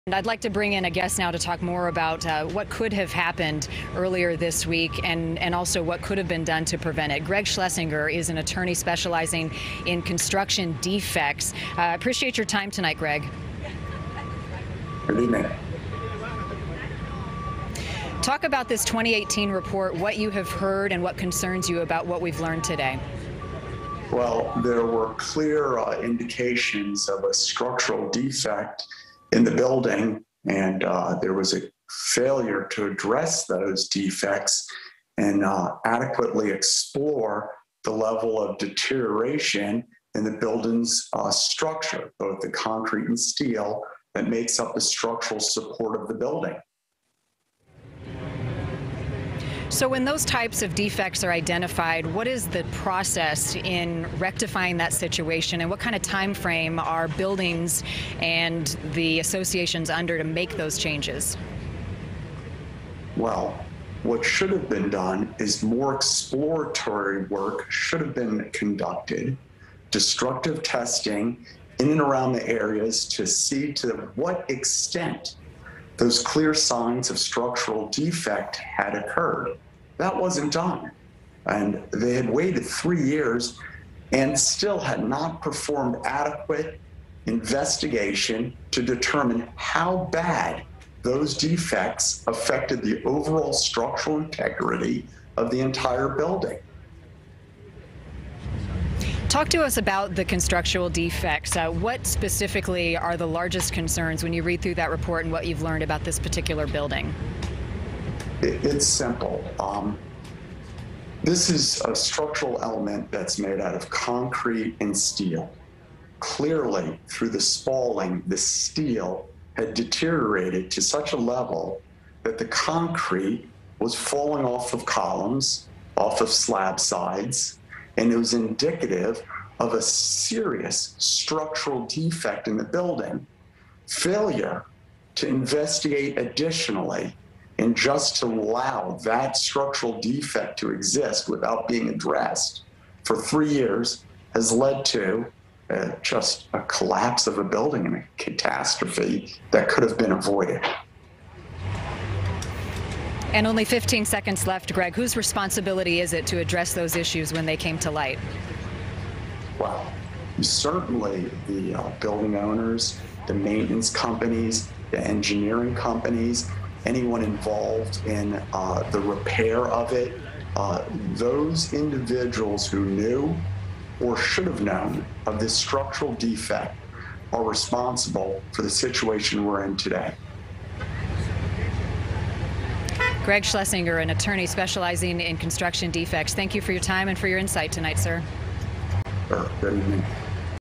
2018 report showed 'major' damage before Florida condo collapse
Officials don't know what caused a Florida beachside condominium tower to suddenly collapse. But experts are closely examining a 2018 report that identified numerous issues with the building.